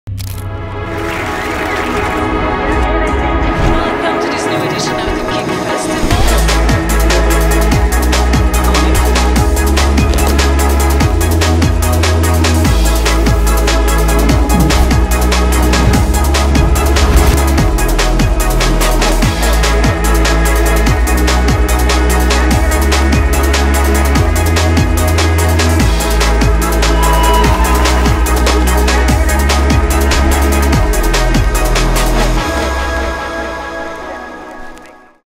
KIKK FESTIVAL 2024 - OCT. 24-27
This 13th edition of KIKK festival, an international festival of digital and creative cultures will take place in Namur from 24 to 27 October 2024!